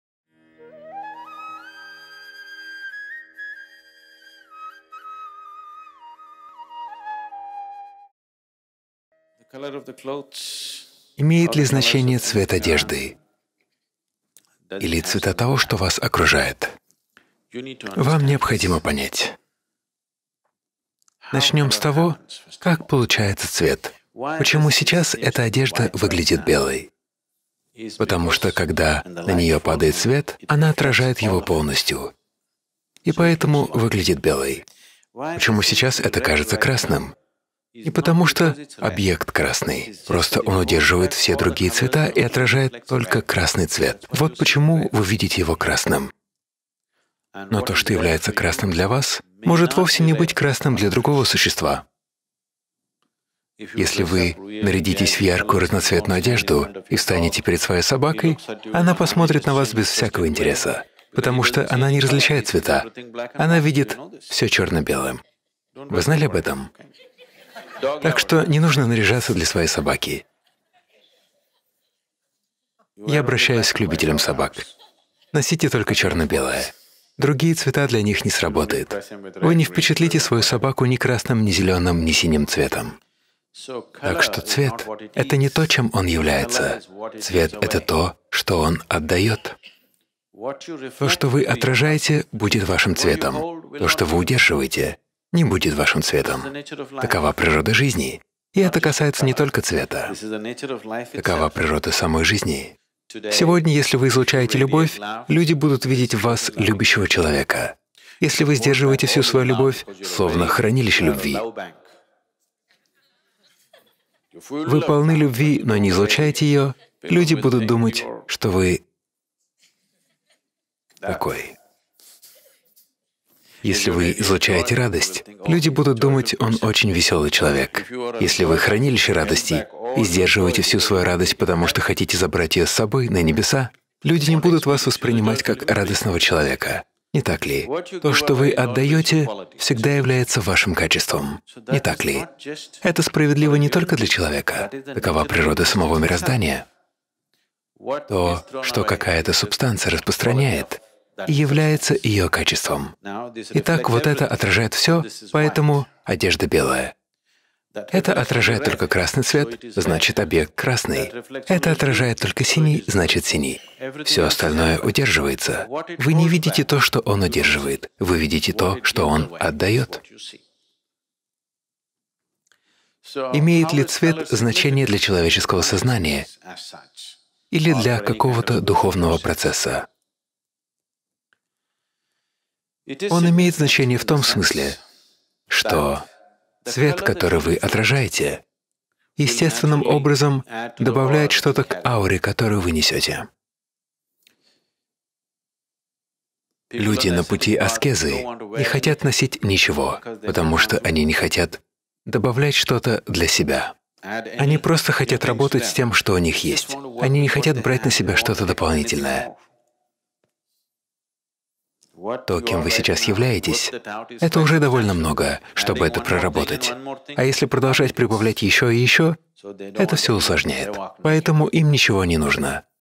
Как выбрать свой цвет?
Замечали ли вы, что цвет одежды может влиять на вас? В этом видео Садхгуру рассказывает о разных цветах и о том, как они могут воздействовать на нас.
 Садхгуру — официальный канал на русском